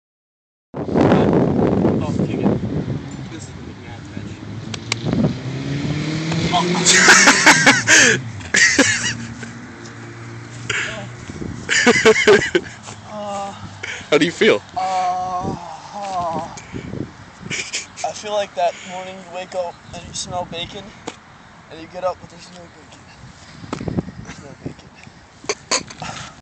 The Mcnab throw!!!
Football from 50 feet away to nuts